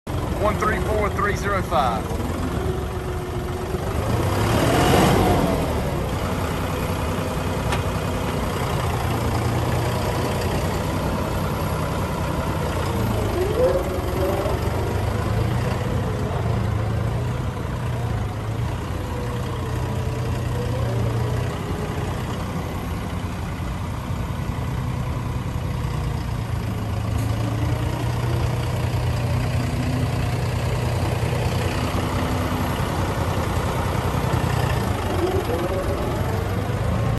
JOHN DEERE 5045E For Sale
ROLLBAR, CANOPY, MFWD, COMPLETE 3PT, DRAWBAR, 1 SET HYD REMOTES, SN- 1PY5045EHF4114650 (HRS SHOW - 840)

 [JOHN DEERE 5045E TRACTOR]